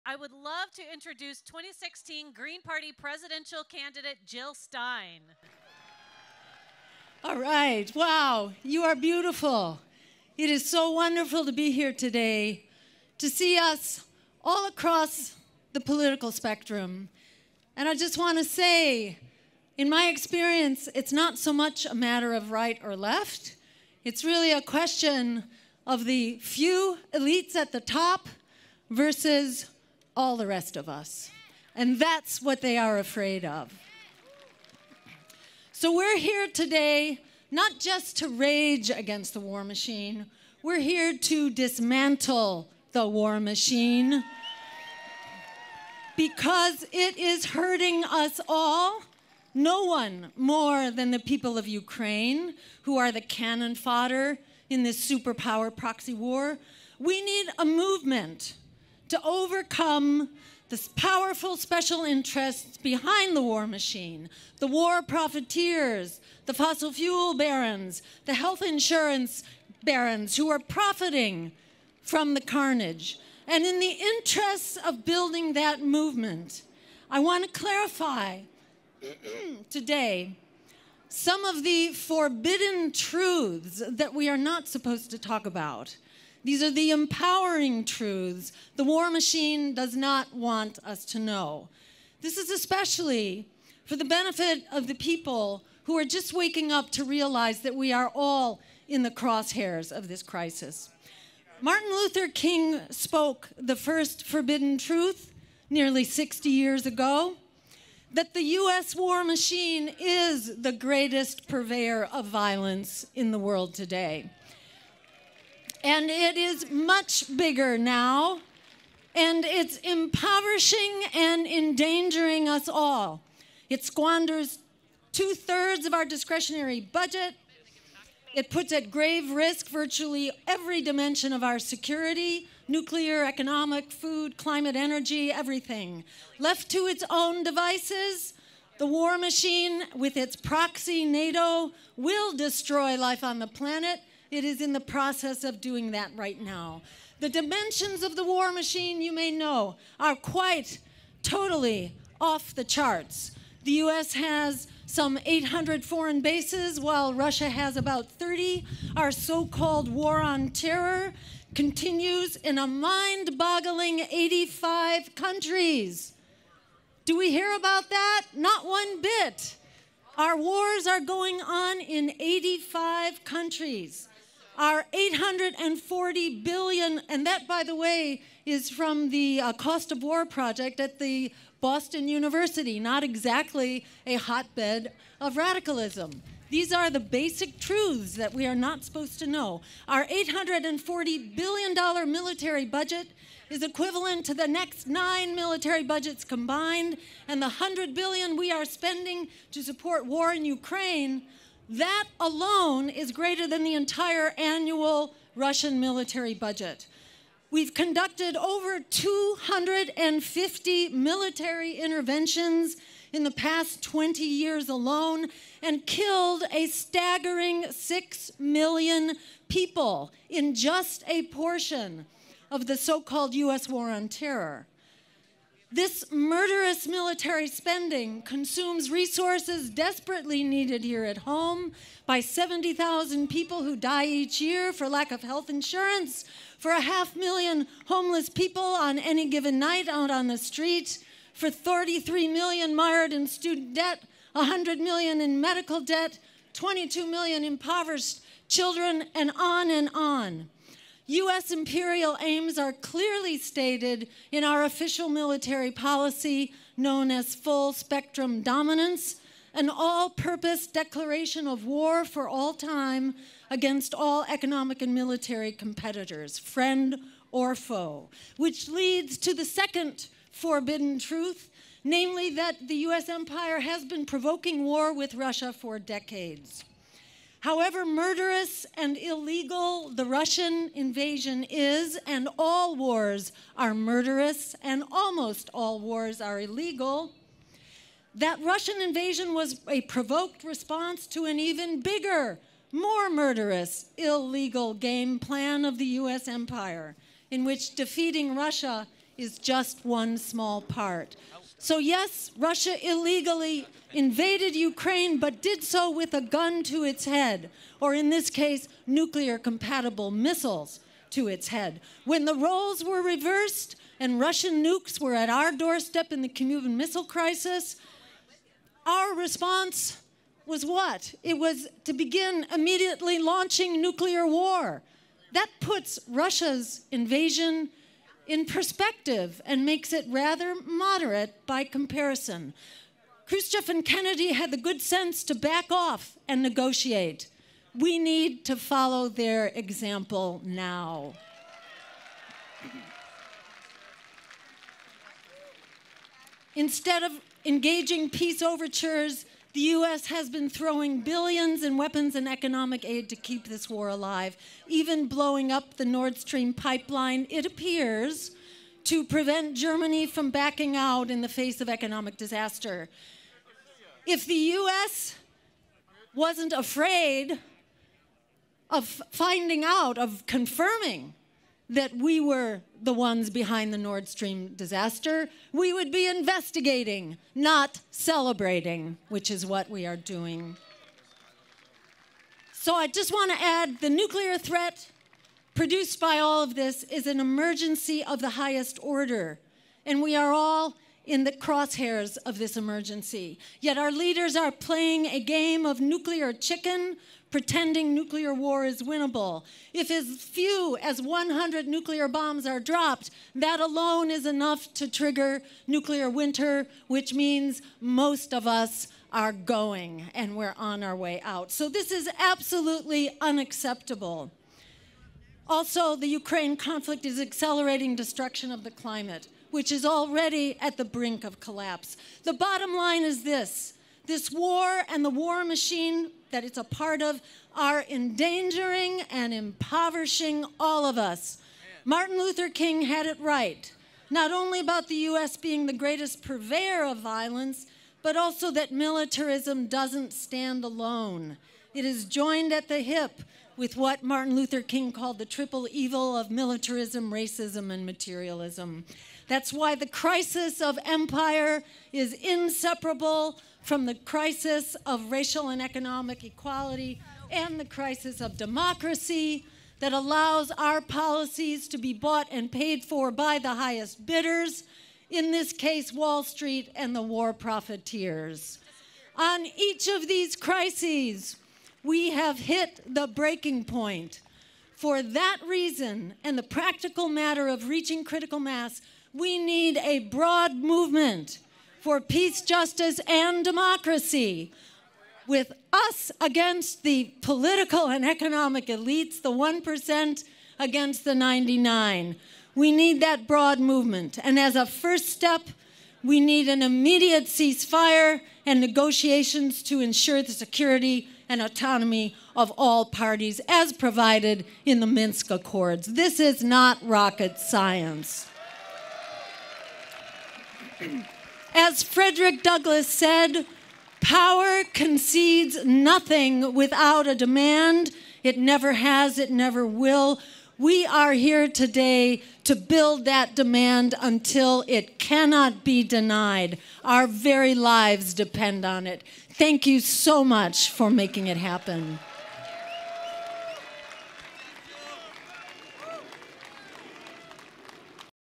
Jill Stein - Rage Against the War Machine
"The US has some 800 foreign bases... Our so-called War on Terror continues in a mind-boggling 85 countries... We've conducted over 250 military interventions in past 20 years... & killed a staggering 6 million people."
Dr Jill Stein, Physician & Presidential Candidate 2012 & 2016